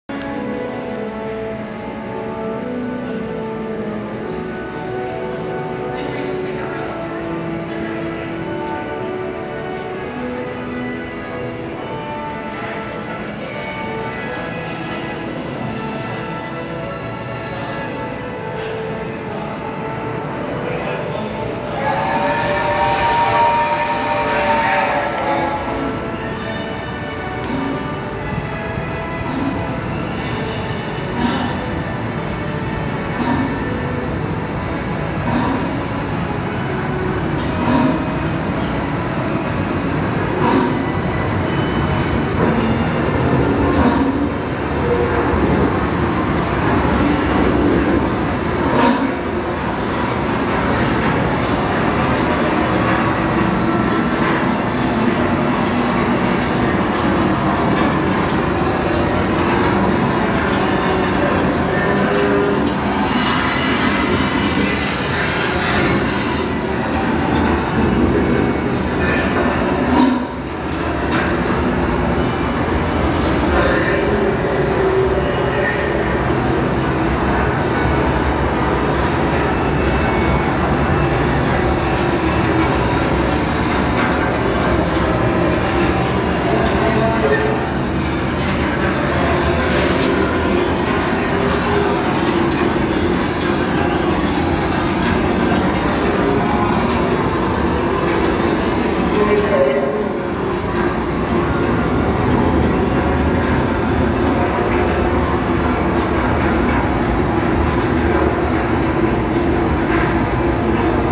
8K Harry Potter Hogsmeade Station Platform Tour in Universal's Islands of Adventure VR180 3D
Come welcome the Hogwarts students on the Hogwarts Express from King’s Cross Station in the London area of Universal Studios Florida to Hogsmeade Station in Universal’s Islands of Adventure after nightfall in 3D VR180. This video is best enjoyed using a VR headset.